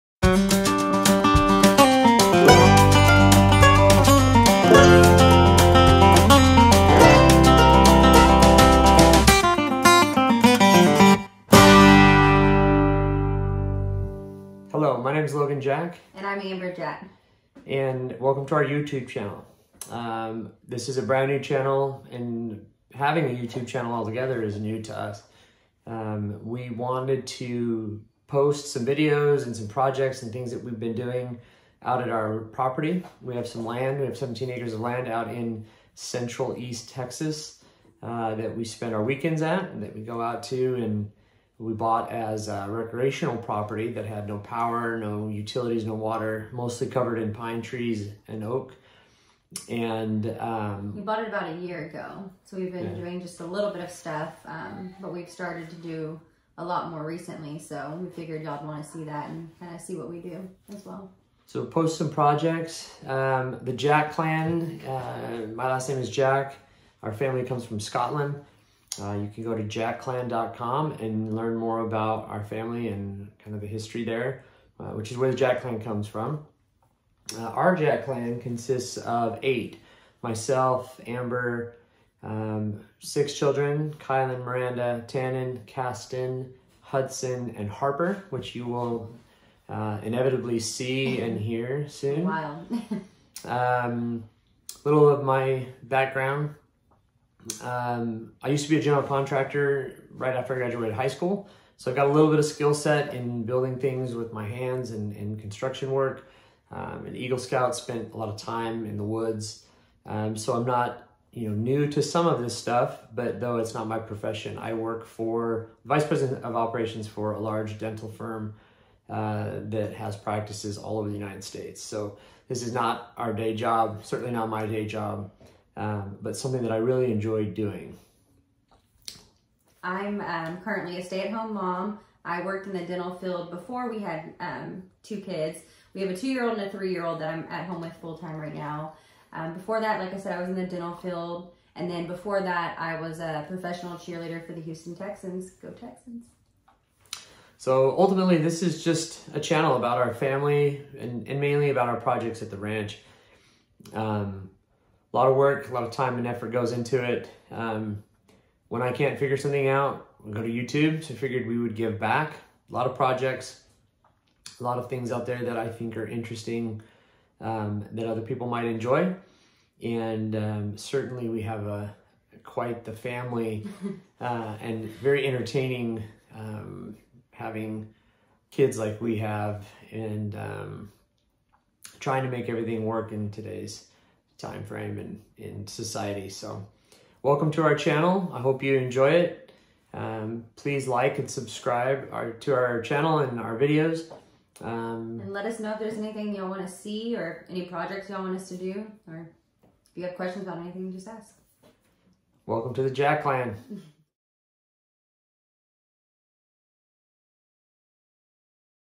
Introduction to Jackclan Ranch
This is an introduction to our channel and a brief about us. Our channel is for homesteaders, weekenders, DIY's and families. We have 17 acres of land that we are developing into our forever home for our family.